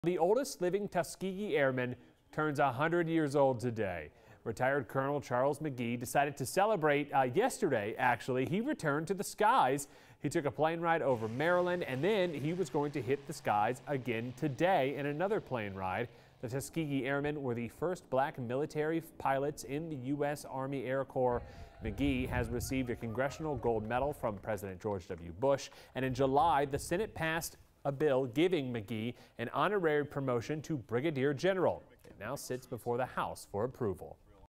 Oldest living Tuskegee Airman turns 100
Retired Colonel Charles McGee celebrated by returning to the skies.